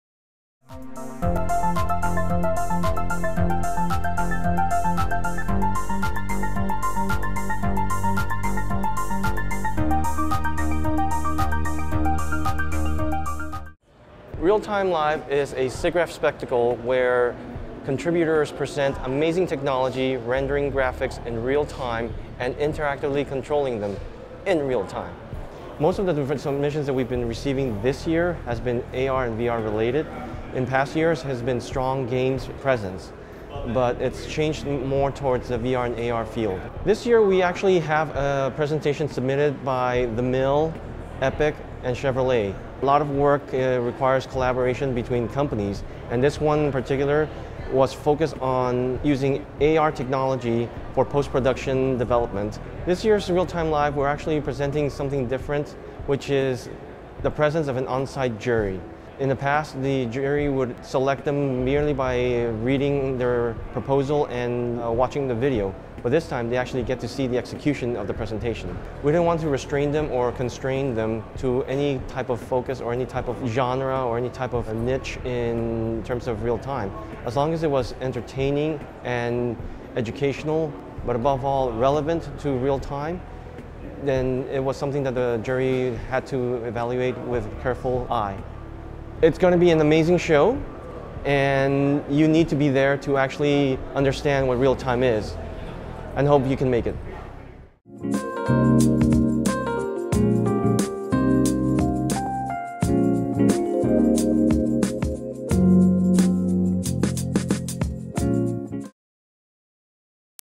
SIGGRAPH 2017 - Real-Time Live!
Real-Time Live! showcases the best of real-time computer graphics during a spectacular evening event at the SIGGRAPH conference. The interactive program at SIGGRAPH 2017 included 10 live presentations demonstrating techniques and tools for technologies across the industry, from VR to mobile and games.